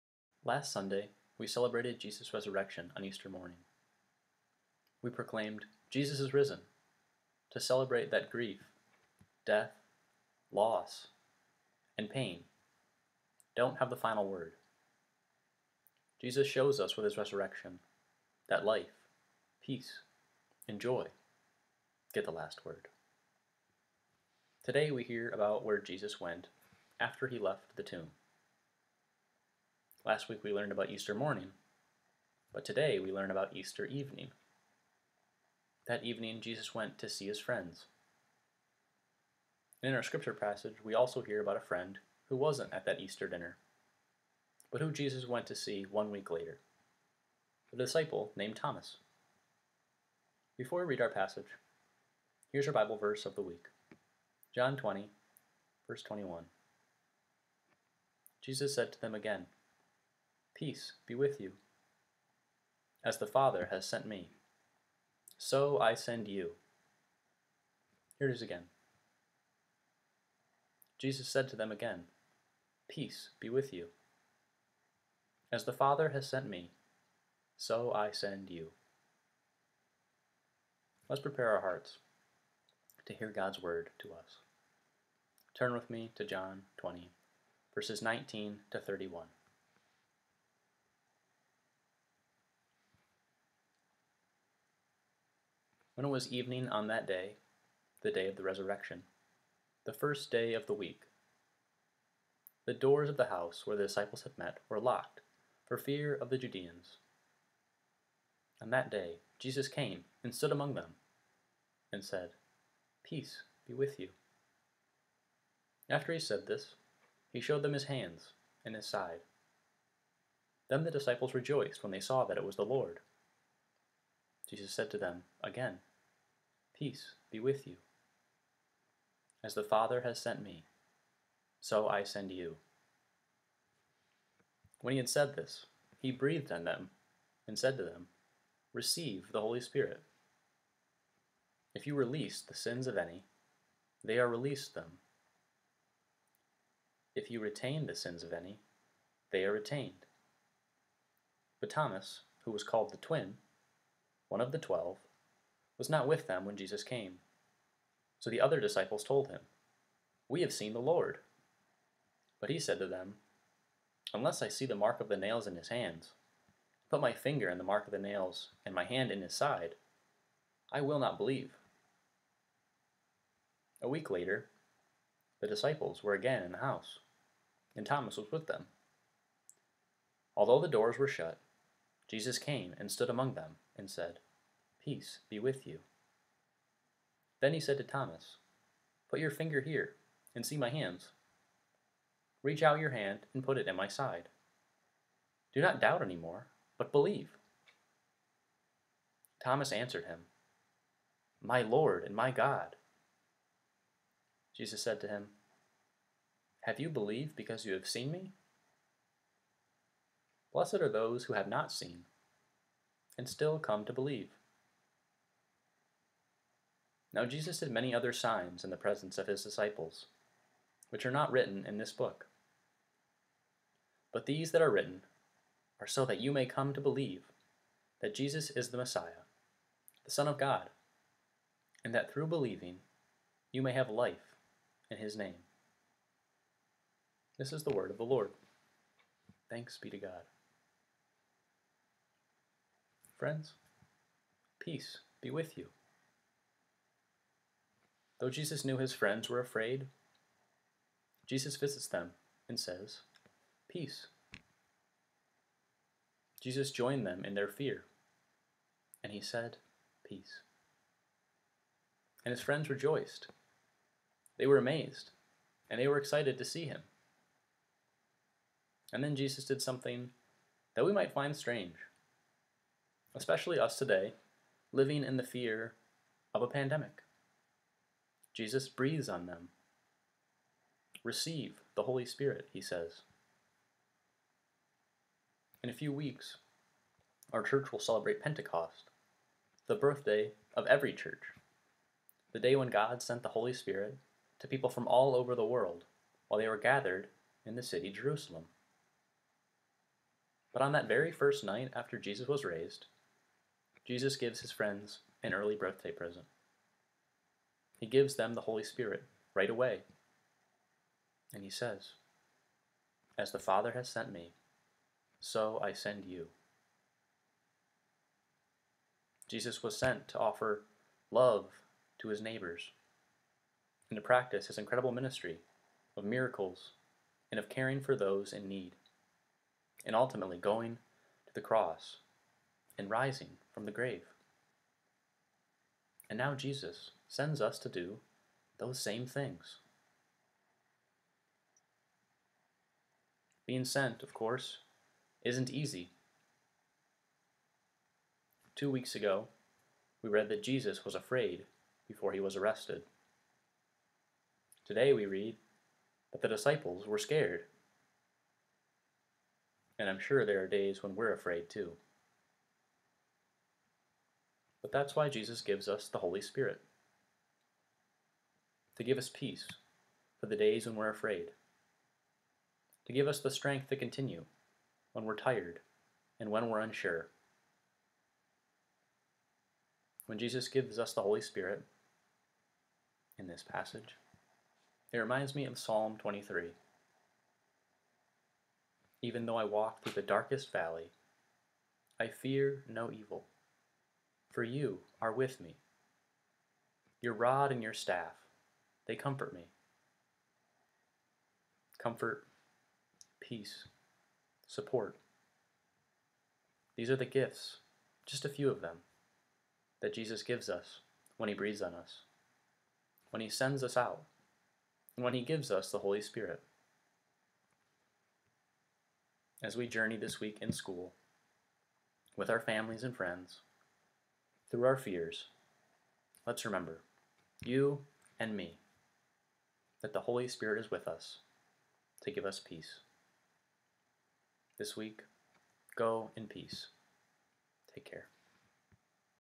"The Gift of the Holy Spirit," 7-9 yr old program
"The Gift of the Holy Spirit"

Church on the Hill Kid's Ministry with Seminary Intern, Tyler Brinks!
7-9 yr old program